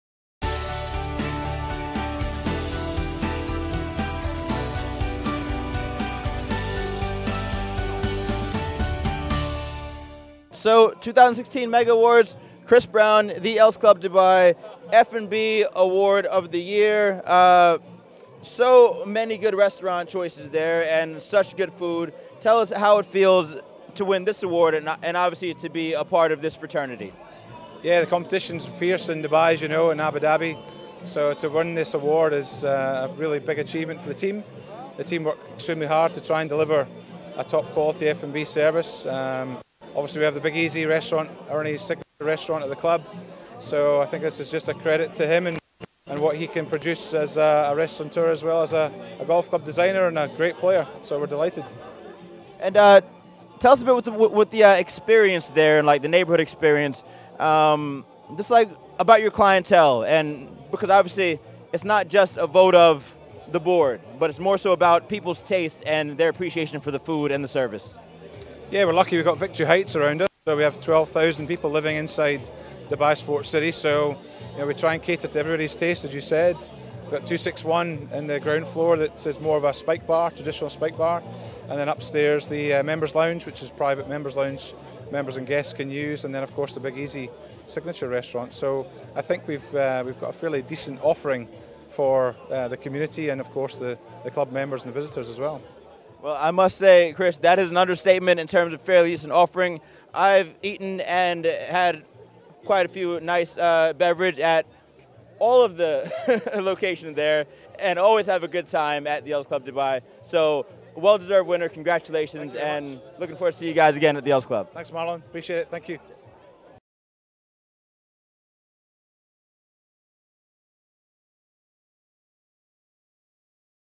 MEGA Awards 2016 Best FandB Team
The Els Club Dubai is well known in the region as a fantastic golf course but it's culinary reputation has now caught up as well.  With a great team that service their 2 unique dining establishments - The Big Easy and 261and not to mention their excellent events catering team, it's no wonder they are the 2016 winners of the Best F&B Team for the Sport 360 MEGA Awards!